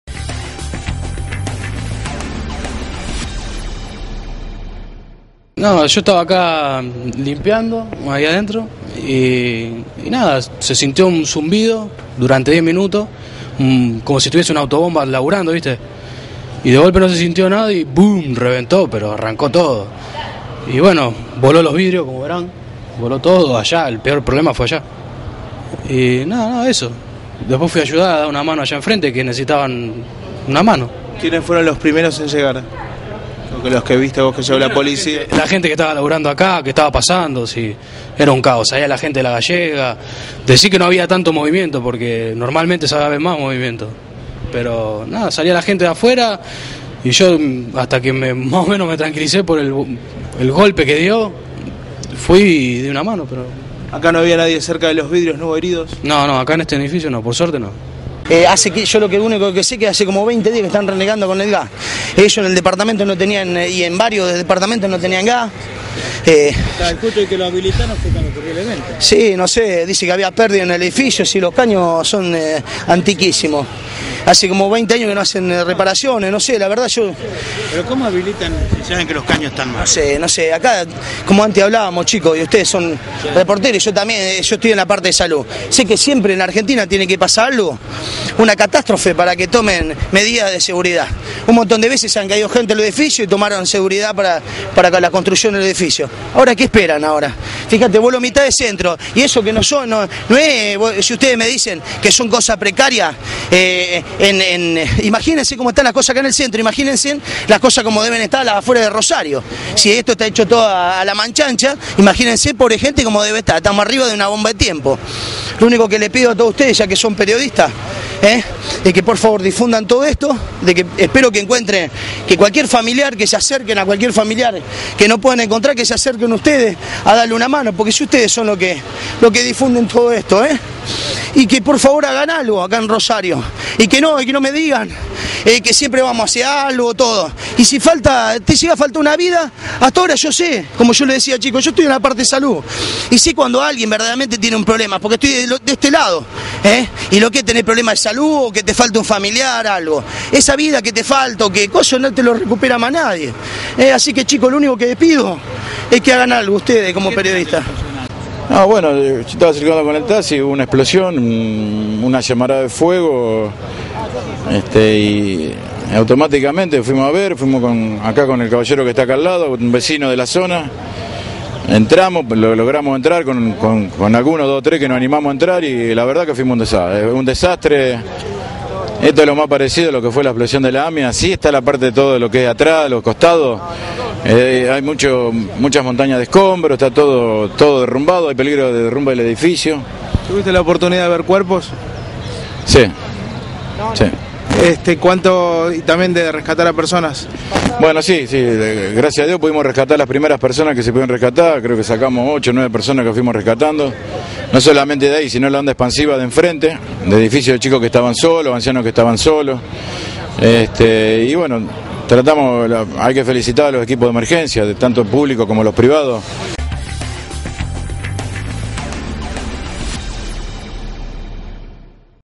EXPLOSION
TESTIMONIOS

El portero del edificio ubicado frente al garaje del supermercado desde donde se realizaron los rescates señaló como se vivió la explosión y los momentos previos.

El hermano de una residente del edificio manifestó su preocupación por no tener novedades del paradero de su hermana.

 Un taxista que pasaba por el lugar brindó ayuda para realizar los primeros rescates, dijo haber vistos cuerpos inmóviles en la zona del derrumbe.